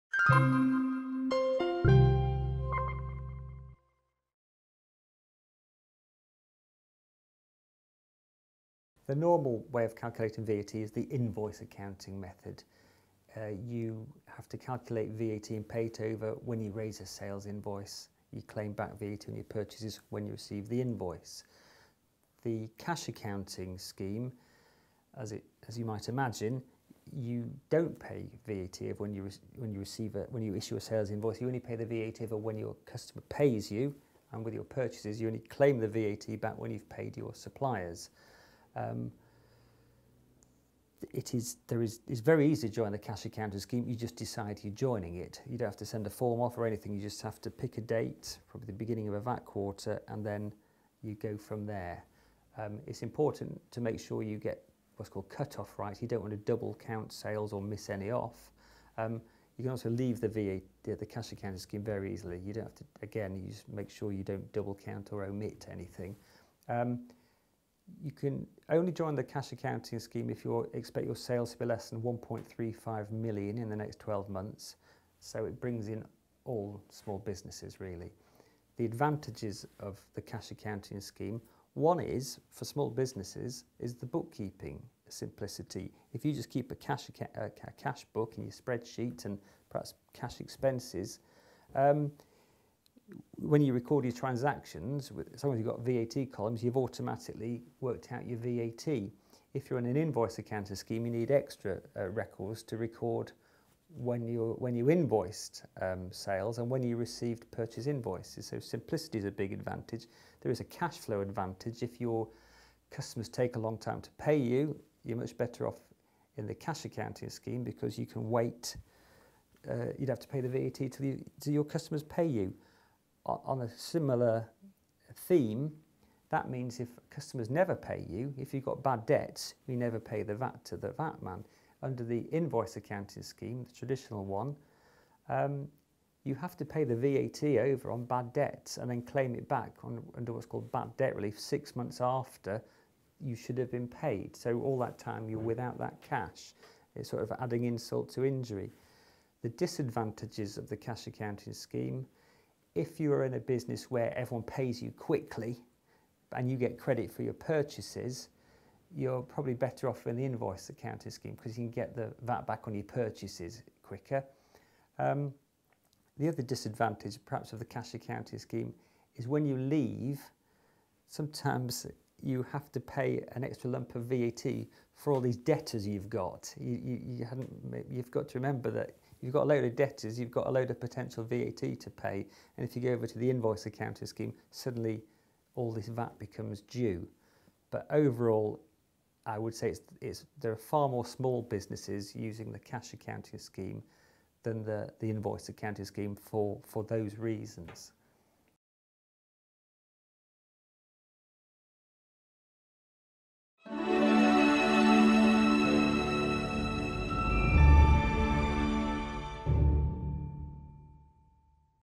Advantages and disadvantages of cash accounting for VAT
VAT is usually calculated when you raise a sales invoice - and you claim back VAT when you receive a purchase invoice.  But, under the cash accounting scheme, you pay over VAT when you receive sales income - rather than when you issue sales invoices – and you claim back VAT when you have paid your suppliers - as opposed to when they invoice you.  It is very easy to join the scheme; just pick a date from the beginning of a VAT quarter.  You can only join the cash accounting scheme if you expect your sales to be less than £1.35m in the next 12 months.

REF: F057